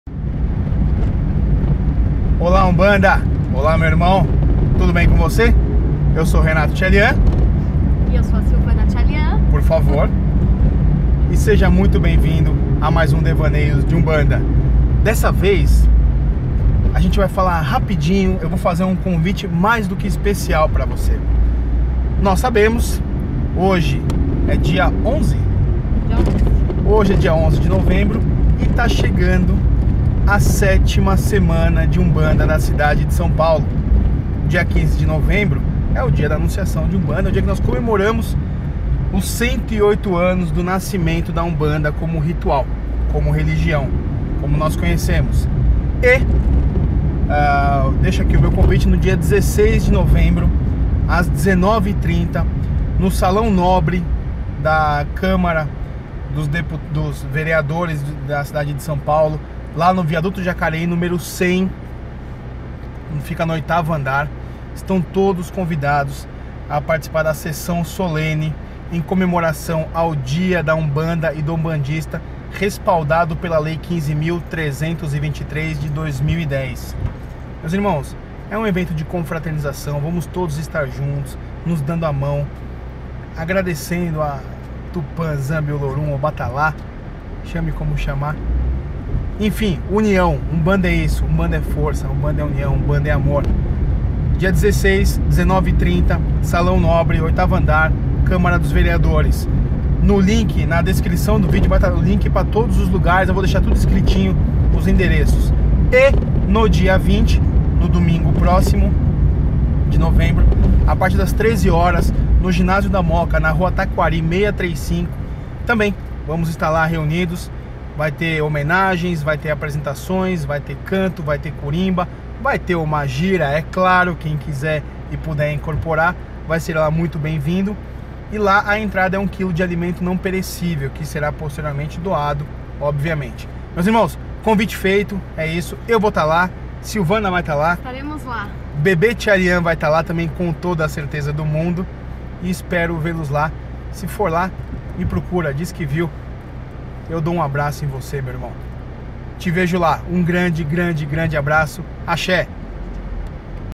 CONVITE - 7ª SEMANA DE UMBANDA DA CIDADE DE SÃO PAULO
Irmãos, conto com a presença de todos na 7ª semana da Umbanda da cidade de São Paulo.

TRAJE BRANCO EM TODOS EVENTOS - PÚBLICO LIBERADO, SÓ CHEGAR!!!

Dia 16/11/2016 as 19:30 - Câmara Municipal de São Paulo
Viaduto jacareí, 100 - 8º andar
Sessão Solene em homenagem a lei 15.323/2010, dia da Umbanda e do Umbandista

Dia 20/11/2016 a partir das 13 horas - Clube Escola Moóca
Rua Taquari, 635 - Moóca
Confraternização, apresentação e gira de Caboclos
IMPORTANTE - ENTRADA 1KG DE ALIMENTOS NÃO PERECÍVEIS.